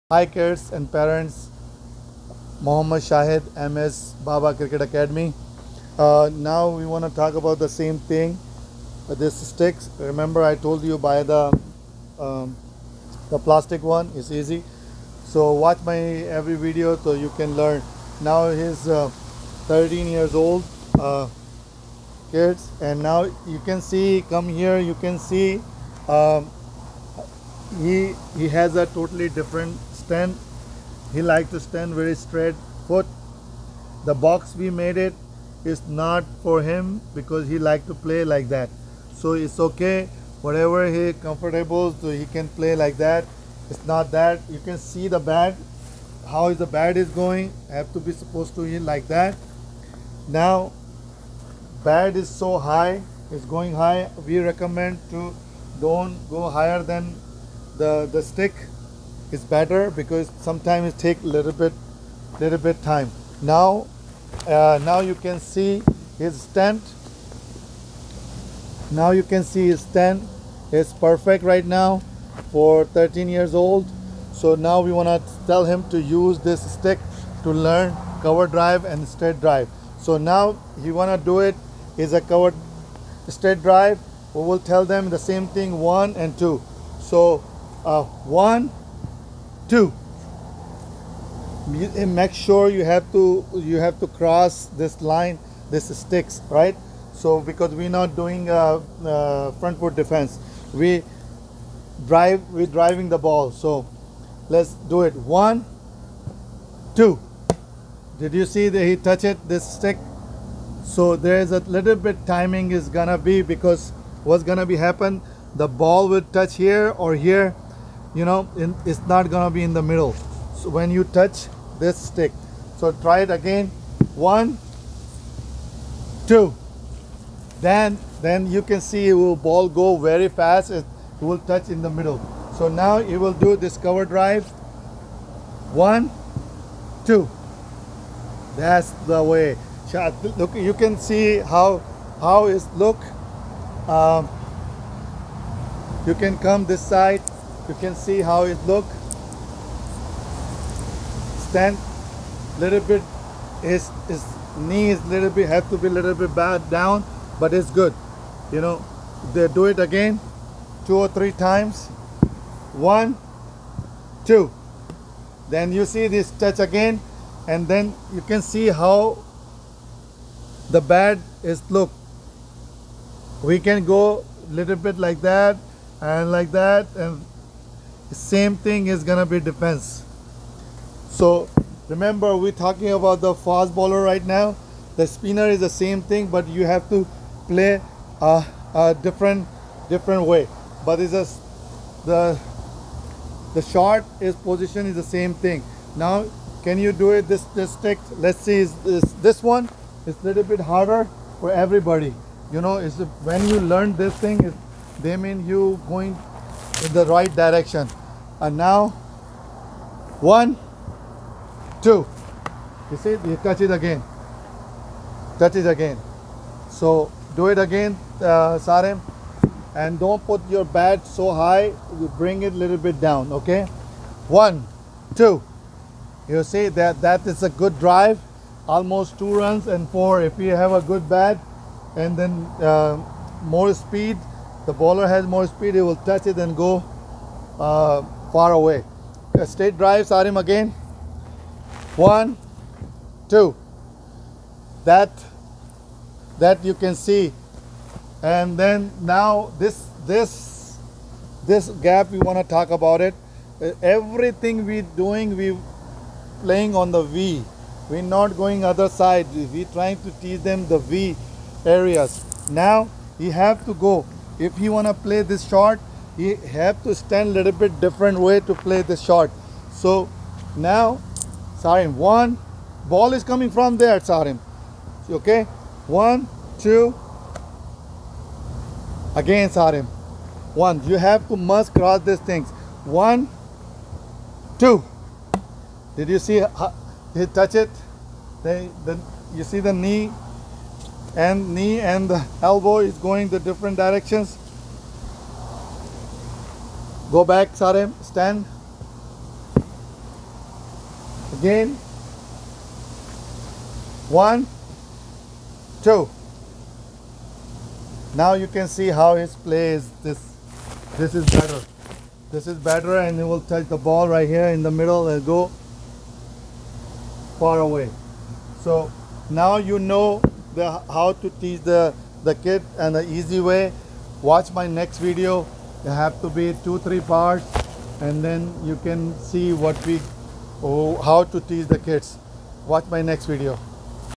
13 Years old learning how to play cricket[new and unique way learn]
new and unique way to learn how to play cricket.13 Years old learning how to play cricket